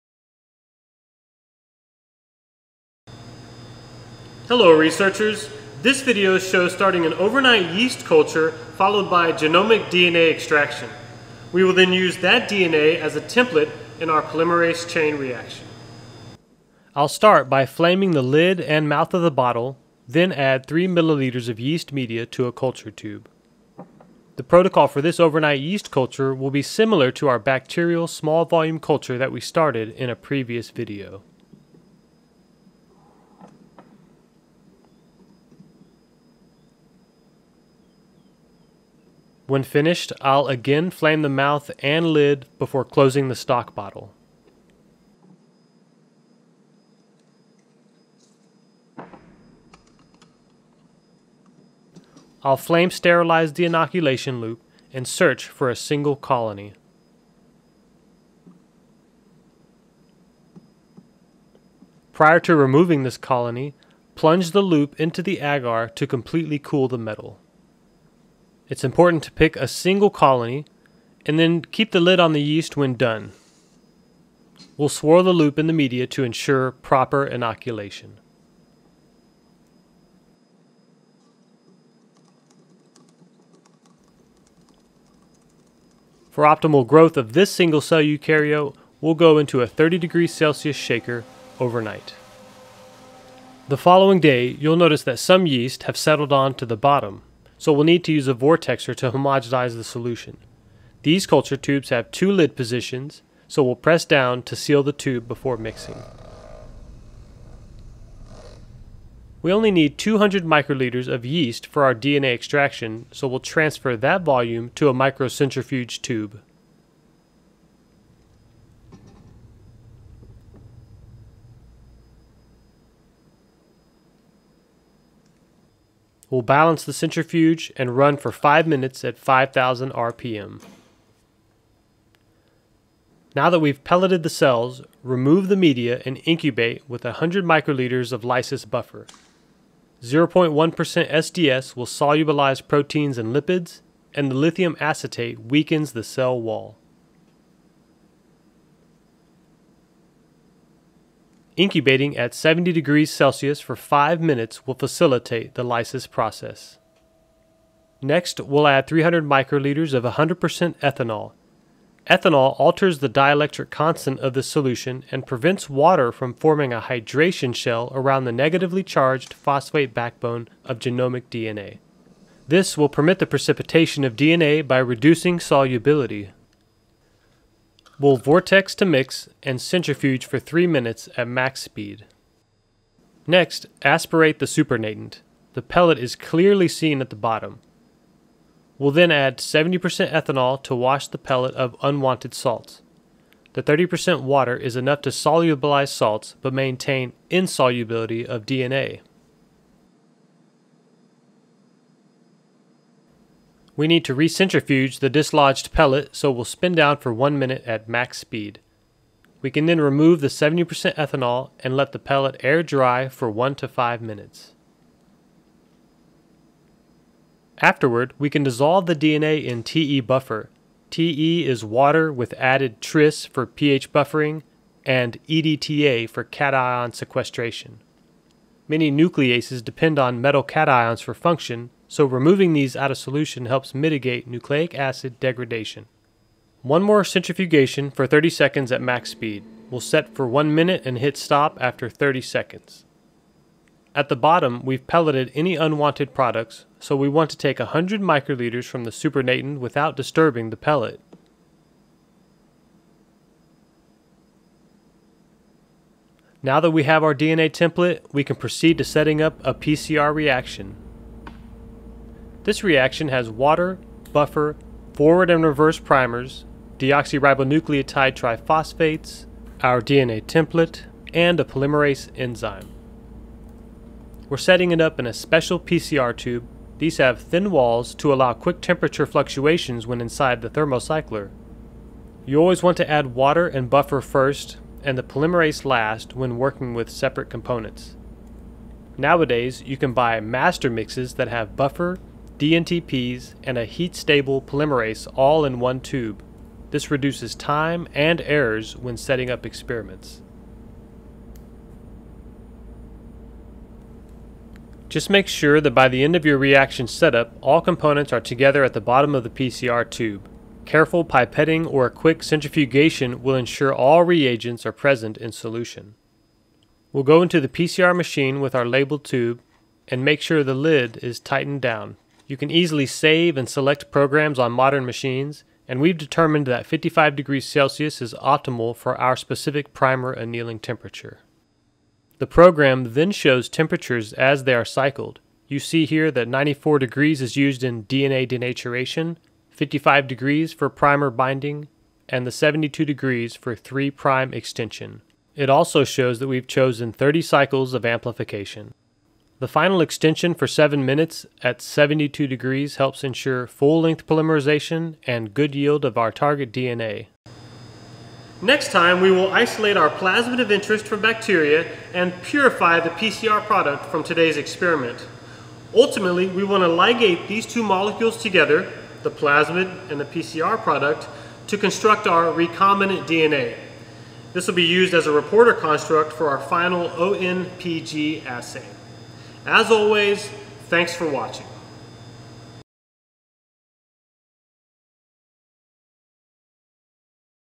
Remote teaching DNA lab: Yeast overnight culture for DNA extraction and PCR
We will start a yeast culture for DNA extraction and amplify the upstream region of the alcohol dehydrogenase 3 (ADH3) gene. The region of interest includes the start site, and the upstream promoter region. We will ligate this into plasmid pLG670Z and place the promoter region of ADH3 in frame with beta galactosidase. After transformation of this recombinant molecule into yeast, we can use it as a reporter construct for ADH3 activation.

Special thanks to Michael Weingart for making these experiments possible.